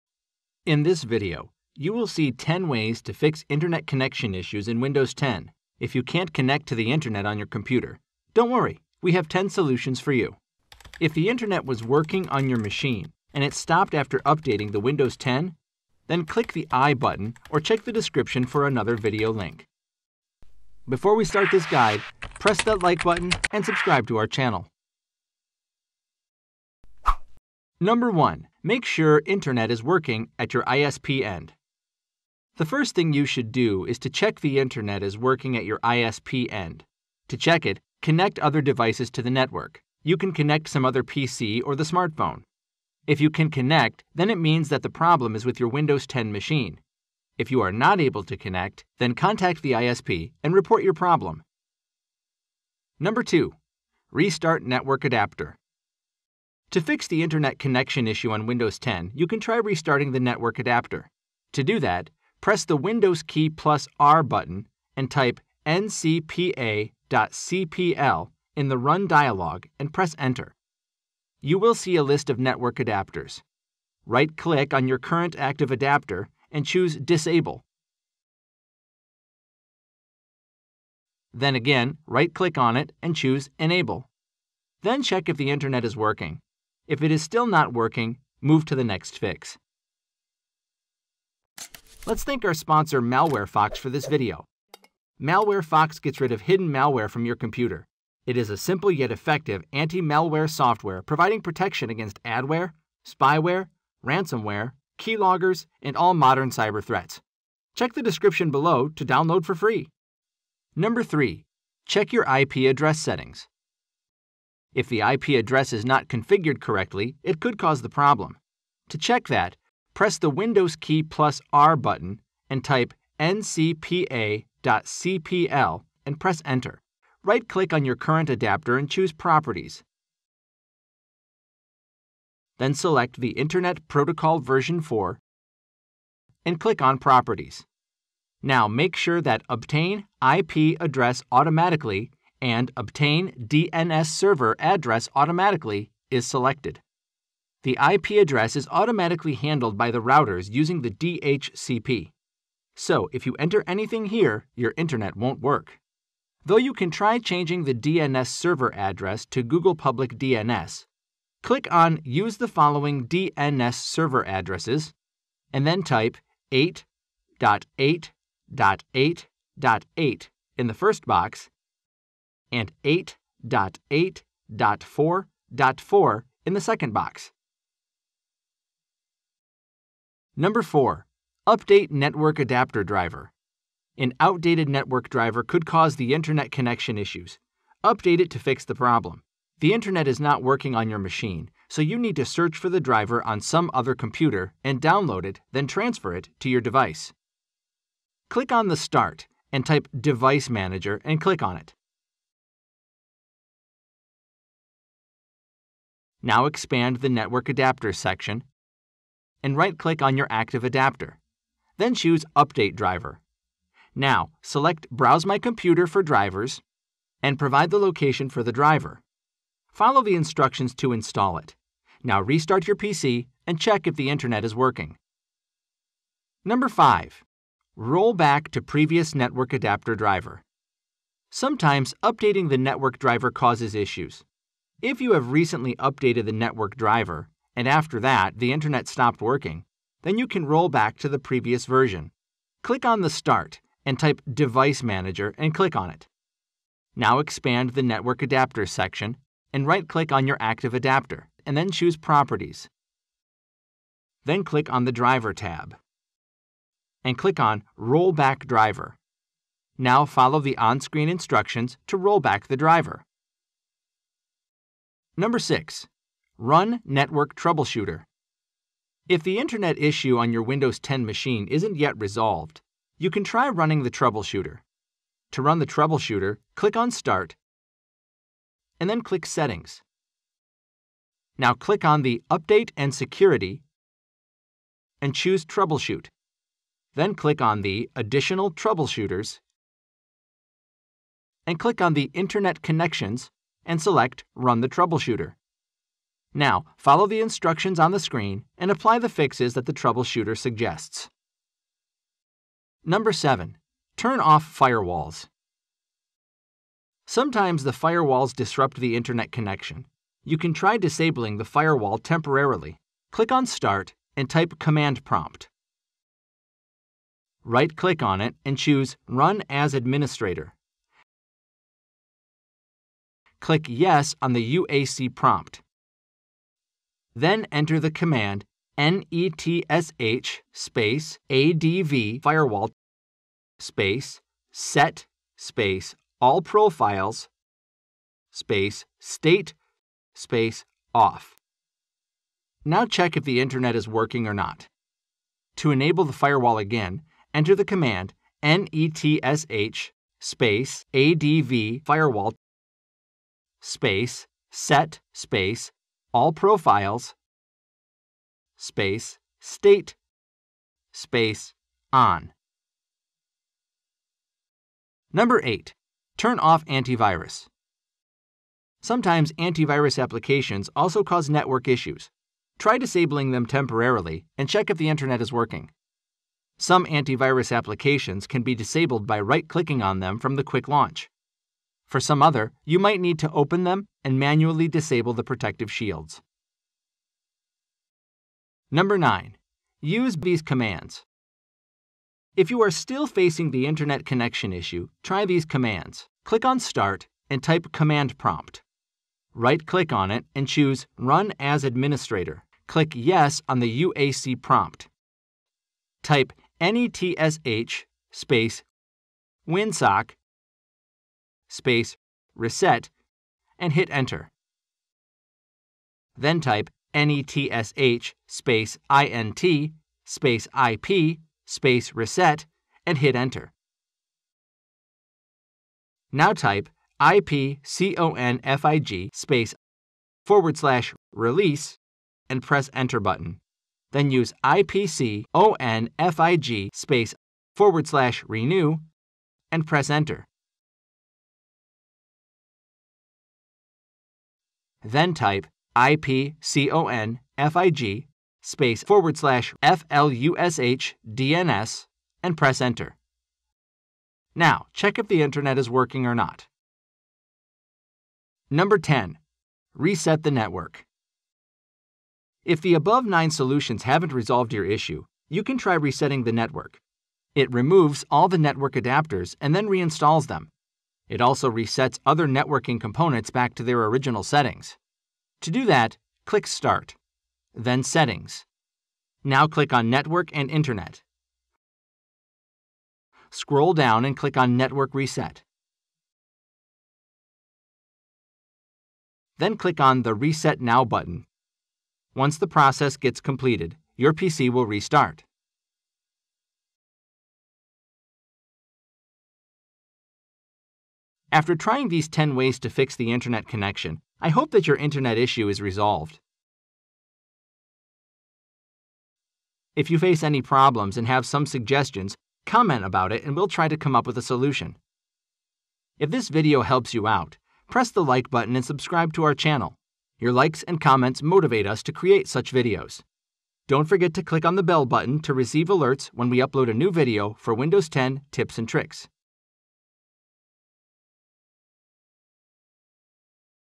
10 Ways to Fix Internet Connection in Windows 10 | Fix No Internet Issue
In this video, you will see 10 ways to fix internet connection issues in Windows 10. If you can't connect to the Internet on your computer, don't worry, we have ten solutions for you.

If the internet was working on your machine and it stopped after updating the Windows 10 then check this video -

The video has the following methods to fix internet connection issues-

1. Make sure the internet is working at your ISP end 0:28
2. Restart network adapter 0:53
3. Check your IP address settings 1:50
4. Update network adapter driver 3:00
5. Rollback to previous network adapter driver 3:46
6. Run network troubleshooter 4:25
7. Turn off firewalls 5:02
8. Turn off antivirus 6:04
9. Use these commands 6:28
10. Reset the network 7:38